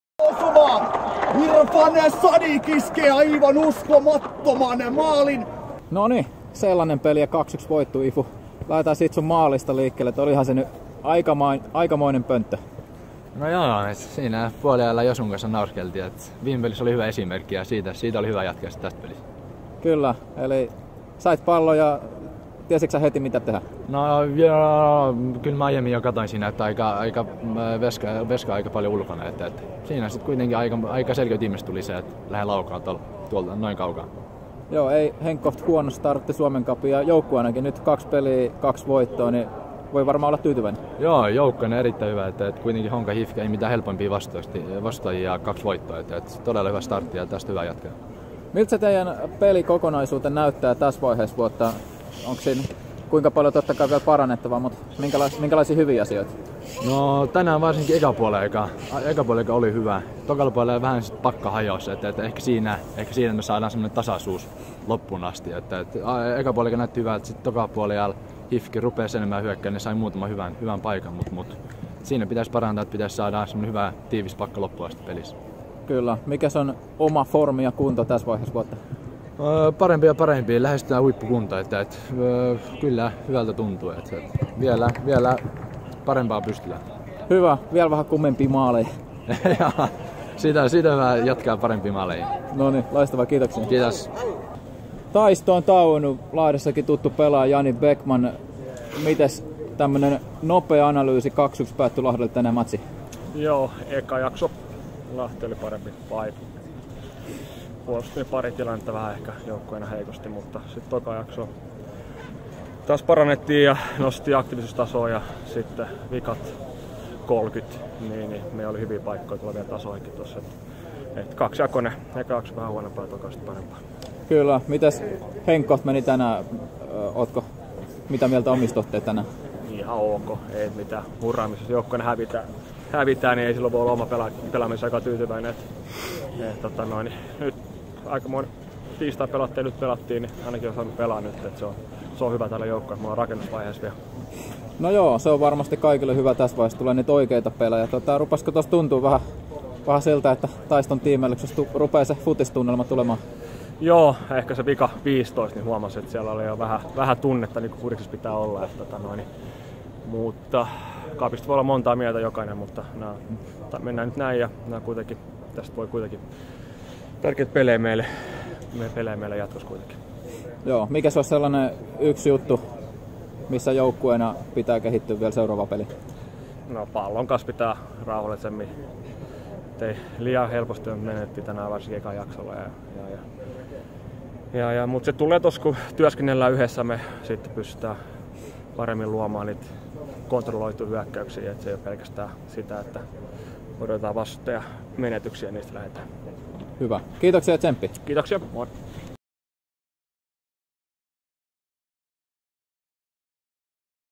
Suomen Cup: FC Lahti - HIFK: Pelaajahaastattelut pelin jälkeen
2.2.2019 FC Lahti - HIFK FT 2-1. Haastattelussa FC Lahden Irfan Sadik ja HIFK:n Jani Bäckman.